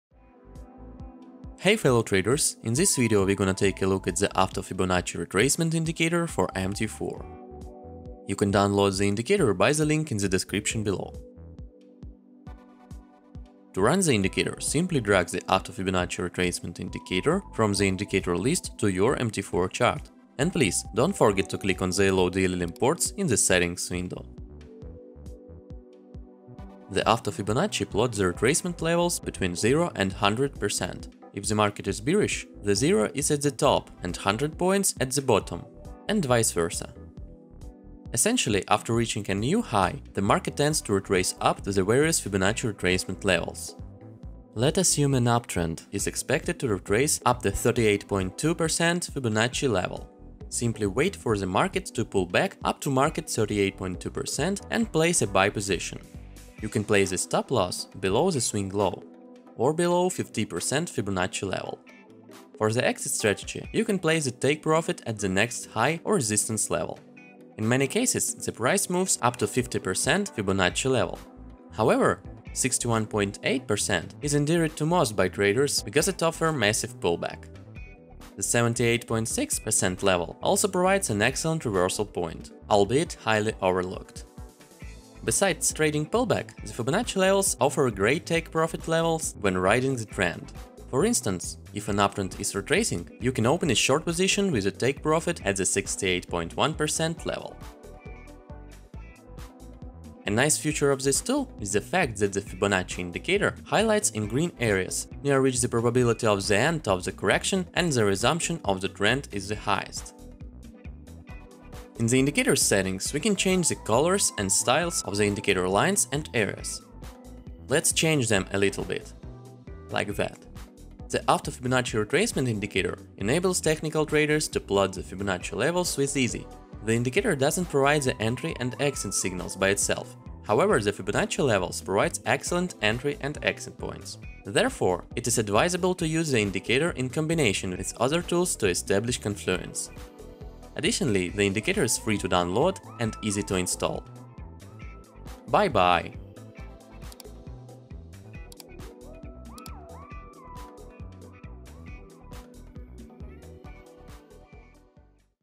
Auto Fibonacci Retracement Indicator for MT4 - OVERVIEW
Auto Fibonacci Retracement Indicator for MT4 - Download FREE

An excellent tool for plotting Fibonacci levels easily. A suitable indicator for perfect trade entry and exits points. As the name implies, the indicator plots the indicator on the chart automatically. It identifies the high and the low of the chart for you. This means you do not have to set the two points explicitly.